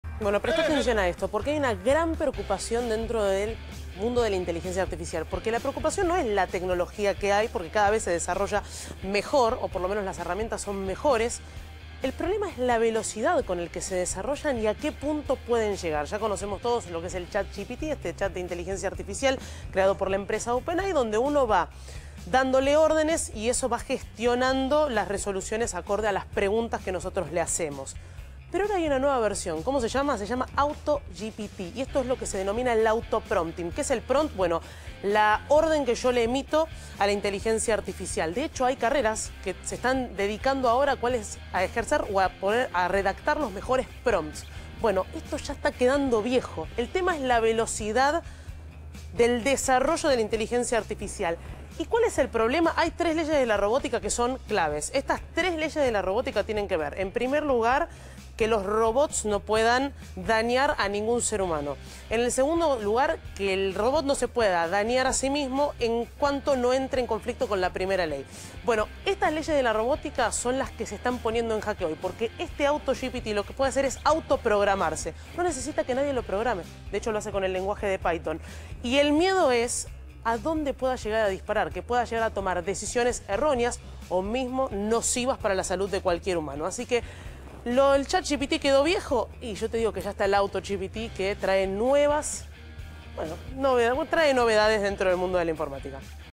🤖AUTO GPT LA "PREOCUPANTE" NUEVA VERSIÓN DE OPEN AI📱
La inteligencia artificial que más rápido aprende preocupa a muchas personas. Que límites puede alcanzar la IA, ¿cuál es su capacidad que todavía no conocemos?